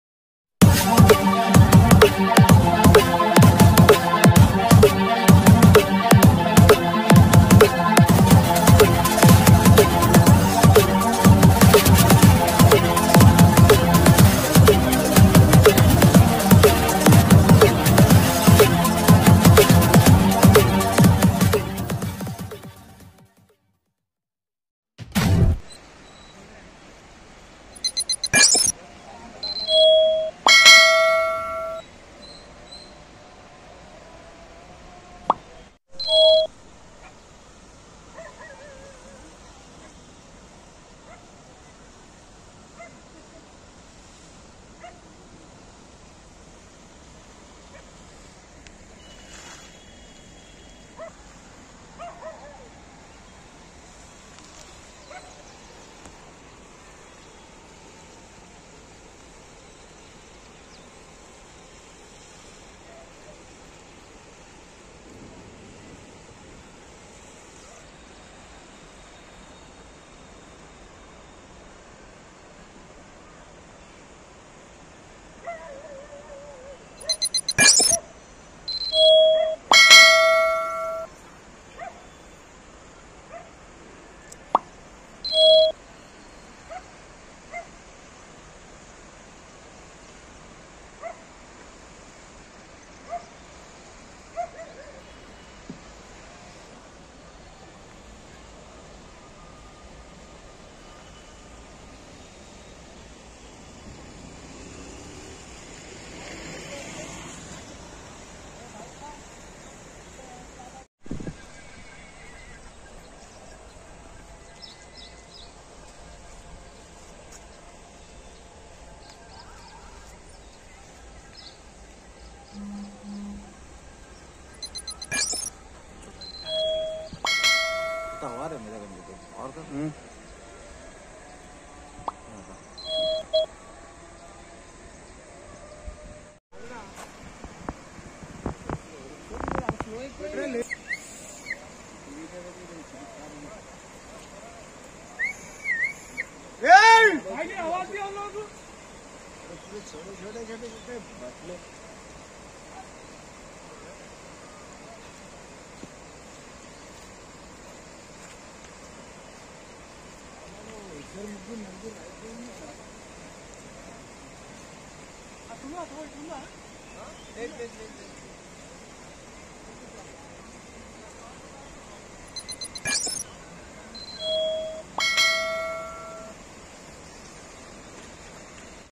MUSI NADDI ME PANI KA BAHAU TEZZ...GANDIPET OR HIMAYAT SAGAR K DARWAZE KHOLDIYE GAYE
MUSI NADDI ME PANI KA BAHAU TEZZ...

GANDIPET OR HIMAYAT SAGAR K DARWAZE KHOLDIYE GAYE HAI WAHAN K LOGAON KO CHAIYE APNE BACCHAON PE NAZAR RAKHEN KOI HADESE HOGAYA TO PARESHANI...